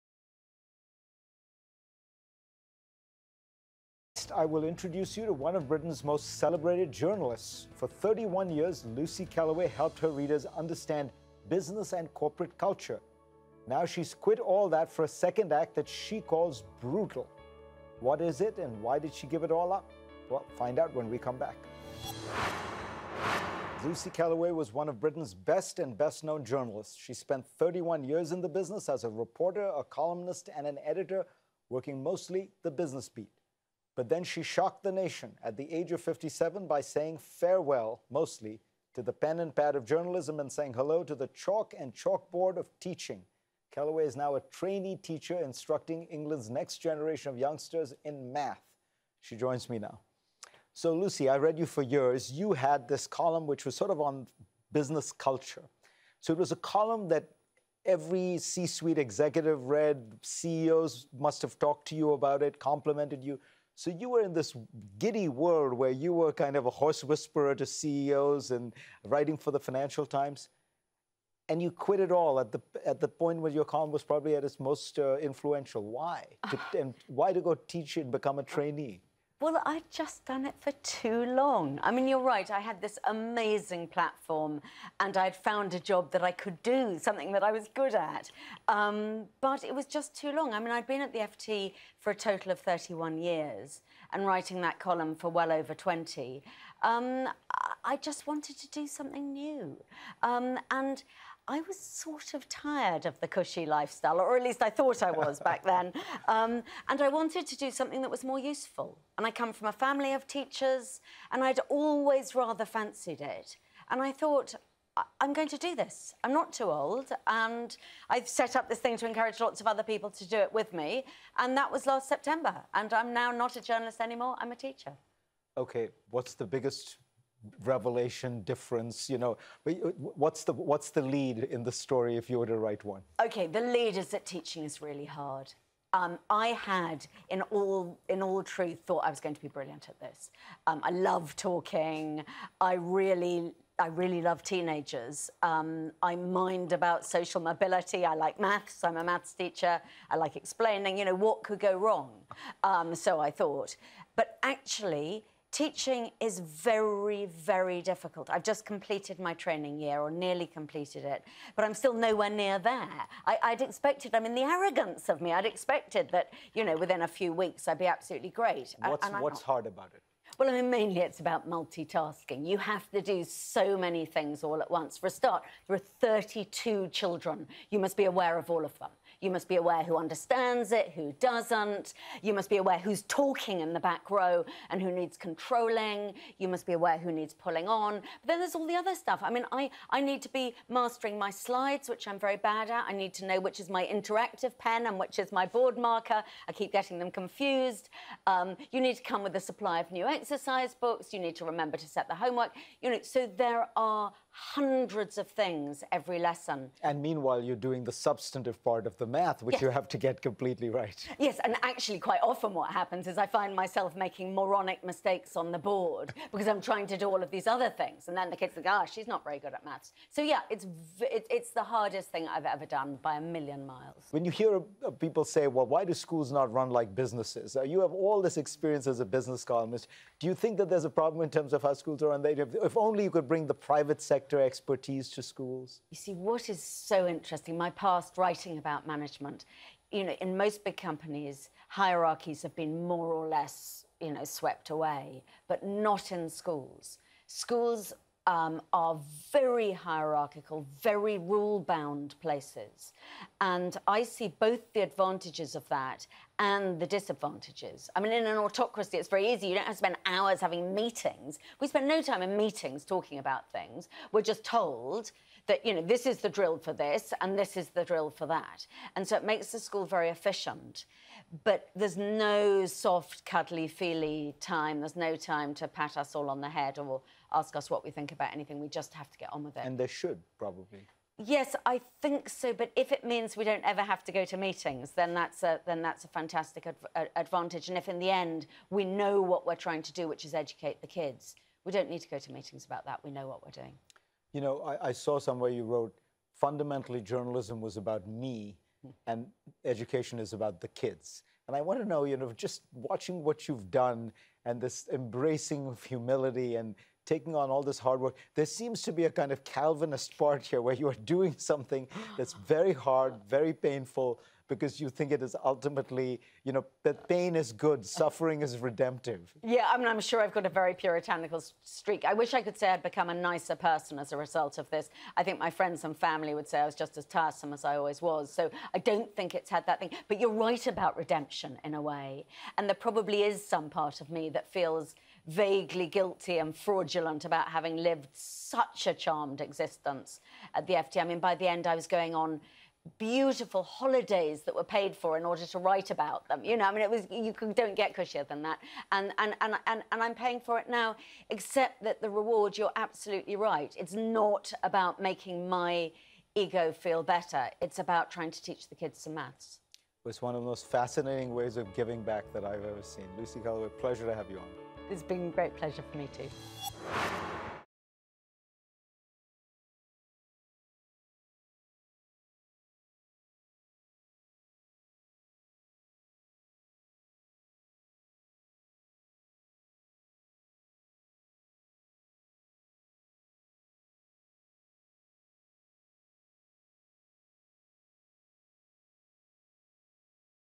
Jornalista torna-se professora
Lucy Kellaway, jornalista do Financial Times, muda de profissão para professora de Matemática